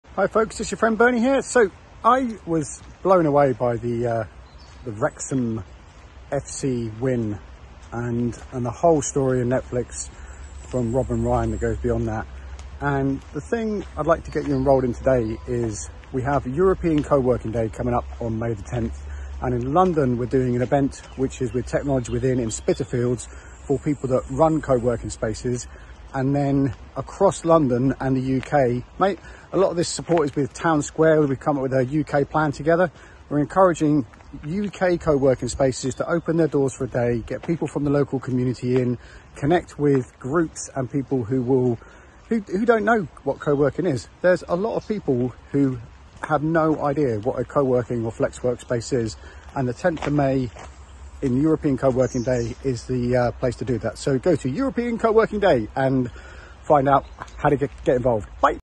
Why are coworking spaces the beating heart of your local economy?
May 10th is European CoWorking Day, and we want to celebrate how coworking spaces add to the communities they are in, not just as a hub but economically, socially and culturally.

What lessons have we learnt since COVID-19 and lockdown?

How do you support local entrepreneurs, micro-businesses and start-ups?

How do you create customers and then support them?

How could your space contribute more to your local community?

We’ve invited three people with a deep and diverse understanding of what “local” means.

Karen West-Whylie - CEO - Barking Enterprise Centre

Vibushan Thirukumar - Co-founder Our Space

Dale Thomson - The Granville - South Kilburn Trust

Hosted by:

Caterina Maiolini - Partnerships & Ops - Judge.me.

Co-created with technologywithin and Urban MBA

Karen has run Barking Enterprise Centre for over a decade and recently opened Three Sixty Workrooms for the local Creator Economy in Barking.

Karens works to support the two spaces’ members, but she has also worked non-stop with the local restaurant owners and other businesses.

Dale has decades of experience working with local London authorities on business support, economic development and, more recently, coworking and flexible workspace via developing the affordable workspace program.

Vibushan co-founded Oru Space, which opened as the COVID pandemic gripped his London neighbourhood.
Since then, Oru Space has gone above and beyond, connecting with people from all walks of life in Dulwich.

To celebrate European Coworking Day, we’ll be streaming this event live with the help of technologywithin and Serviced Office Space Spitalfields I Liverpool Street.

or @bernieJMitchell on IG

European Coworking Day IG: @europeancoworkingday